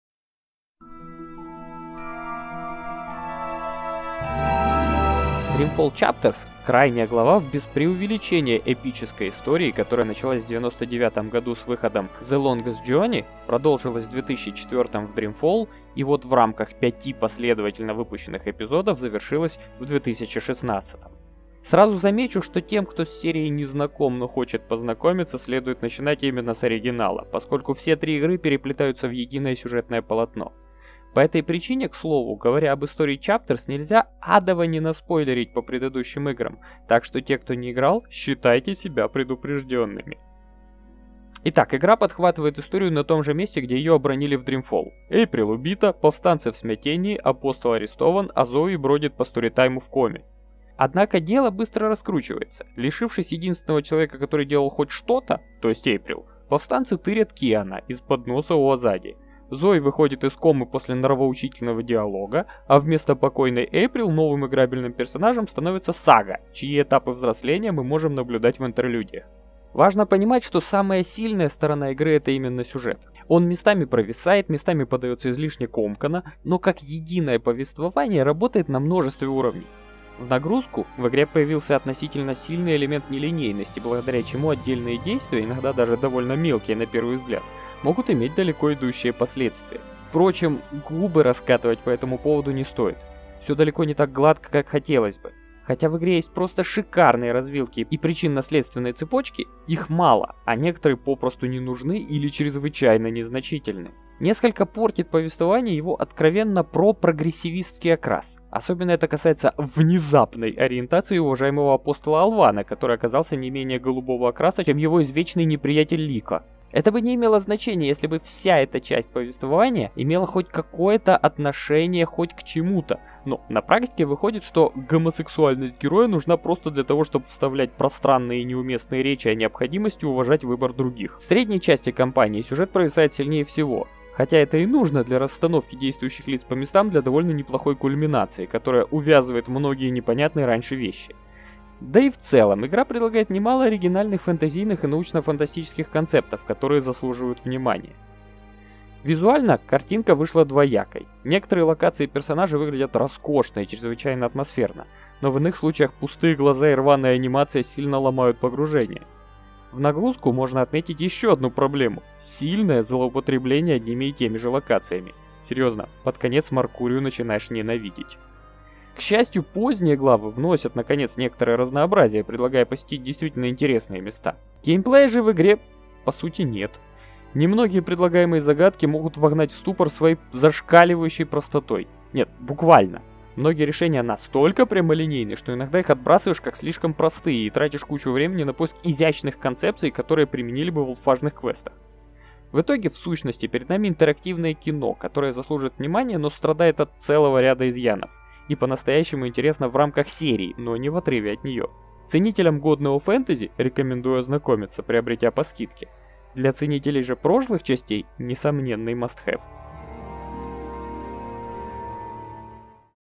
Dreamfall Chapters - Обзорпополной №14
Не идеальное, но достойное завершение истории.